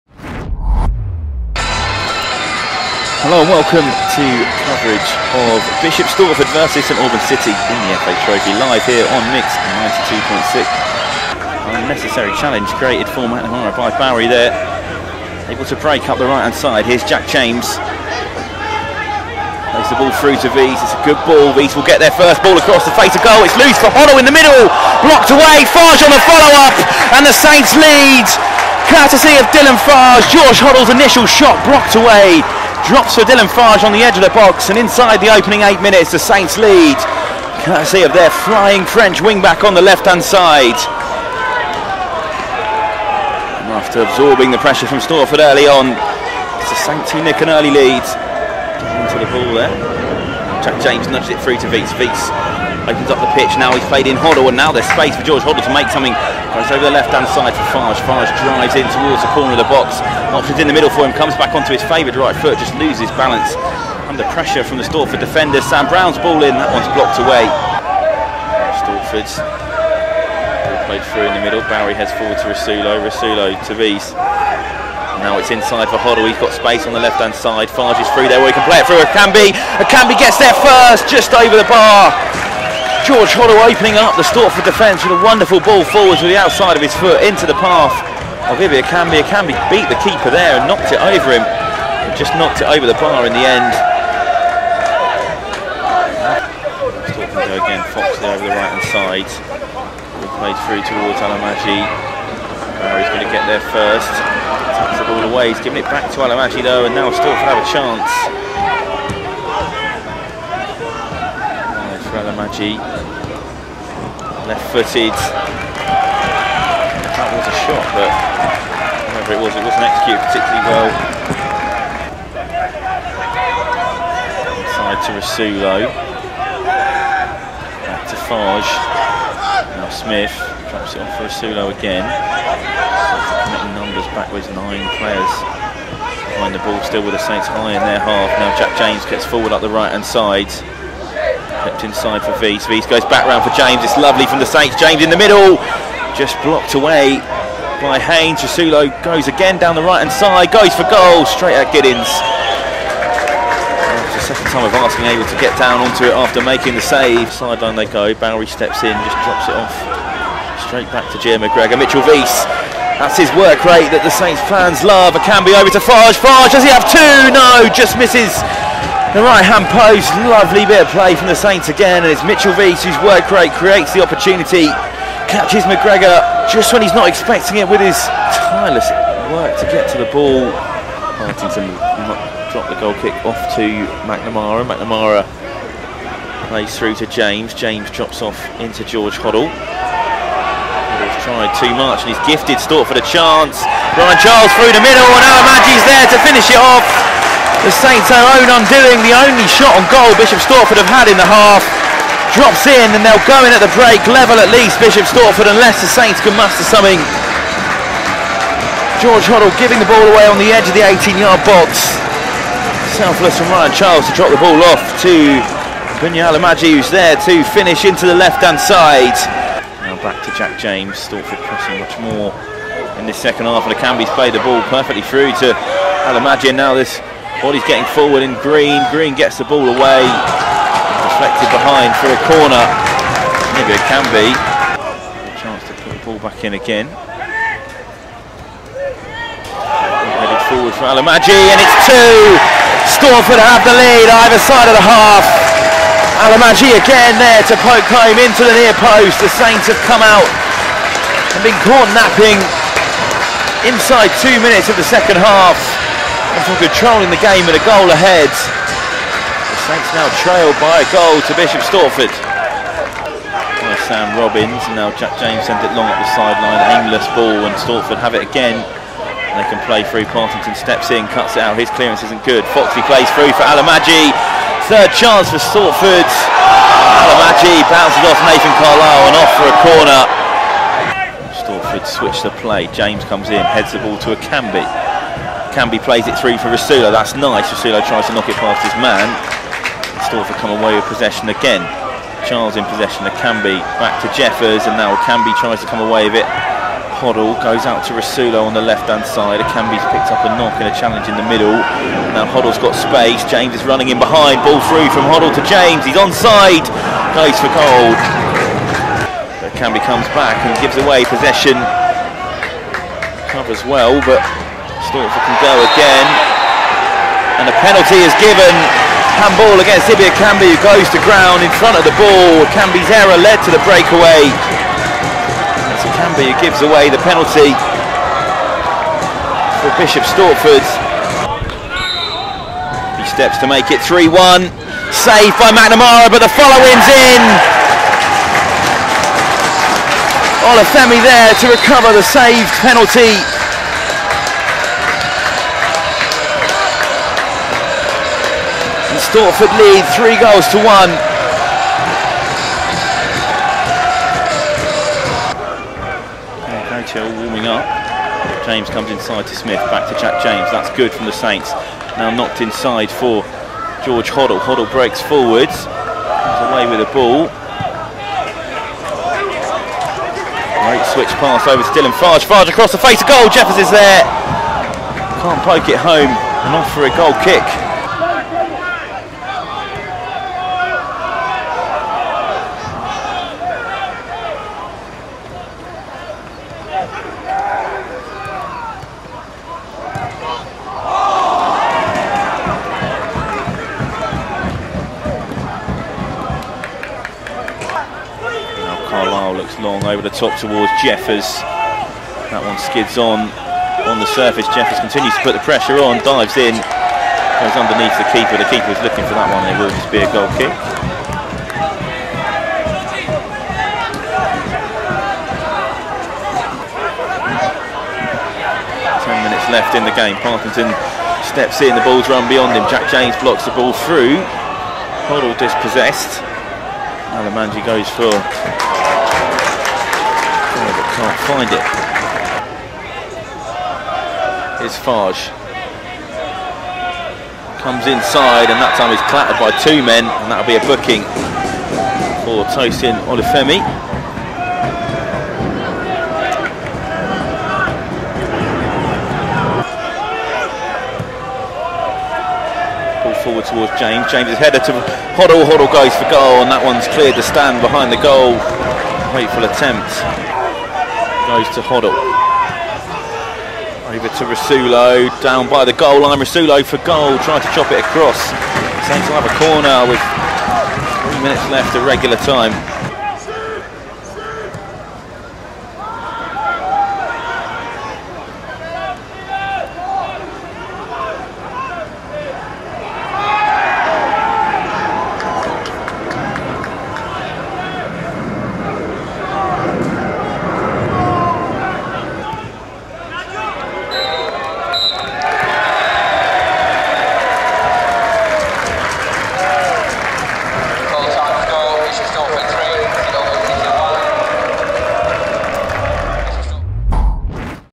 HIGHLIGHTS | Bishop's Stortford vs St Albans City | Isuzu FA Trophy | 18th November 2023
Watch back City's disappointing FA Trophy exit